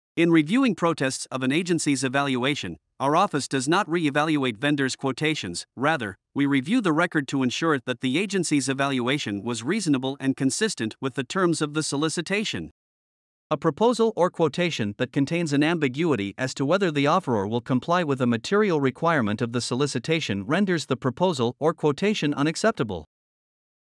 Bid protests: GAO reviews the record.
GAO bid protests - key case law excerpts: FAR 15.305 - Unacceptable or offers not in compliance with solicitation

In reviewing protests of an agency's evaluation, our Office does not reevaluate vendors' quotations; rather, we review the record to ensure that the agency's evaluation was reasonable and consistent with the terms of the solicitation. A proposal or quotation that contains an ambiguity as to whether the offeror will comply with a material requirement of the solicitation renders the proposal or quotation unacceptable.